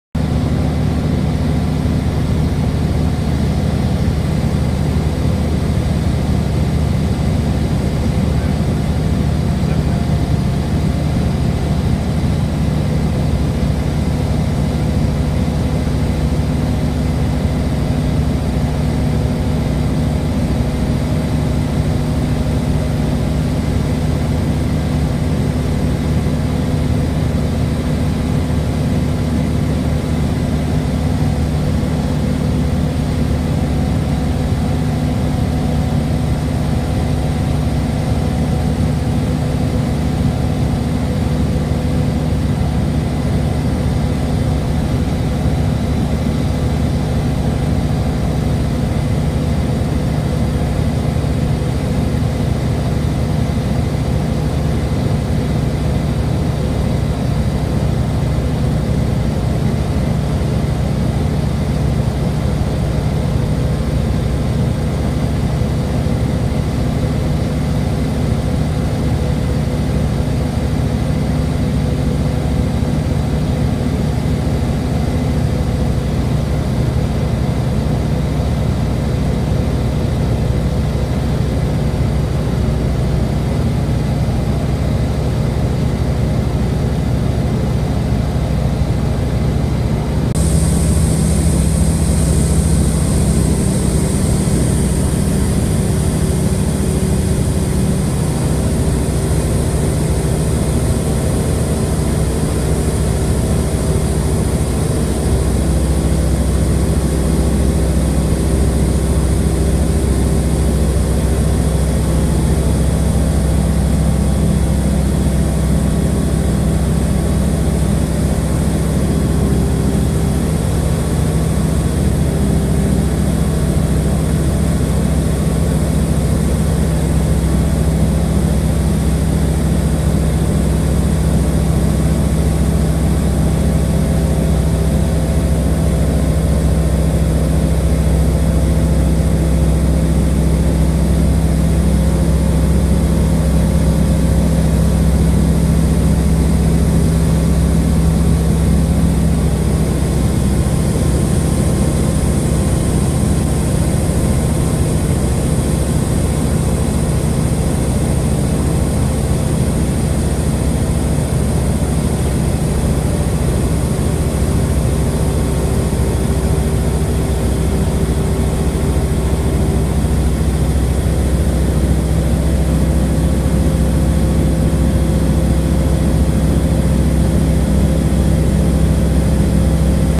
Aerial Footage of Flooding Over Mitchell River and Bairnsdale
Source: East Gippsland Flood Watch